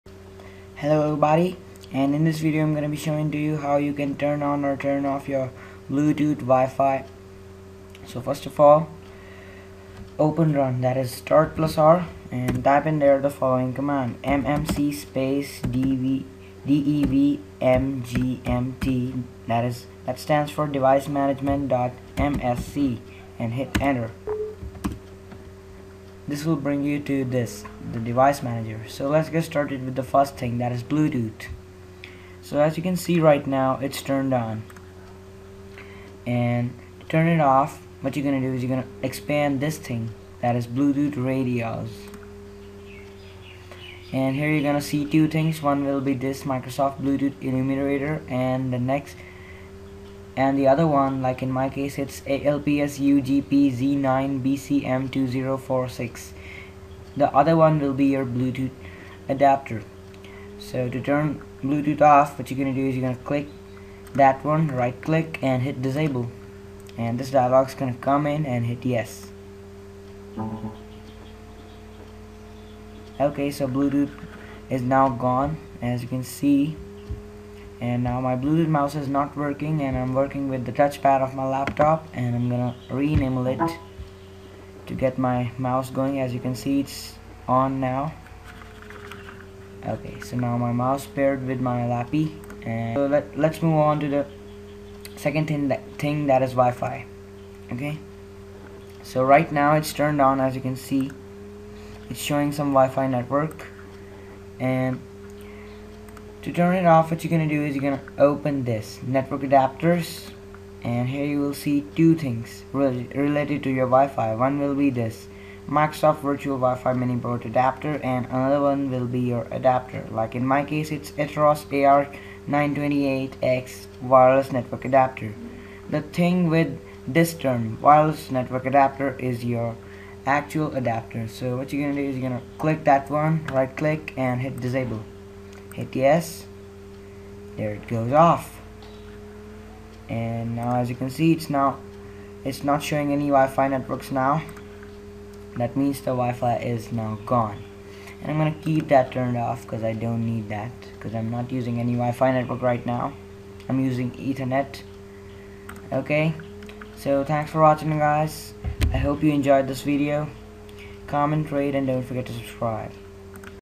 How to turn on or turn off wifi and bluetooth in Windows 7 and Vista
Hi guys,in this video I'll show you how to enable or disable wifi and bluetooth in Windows 7 and Vista.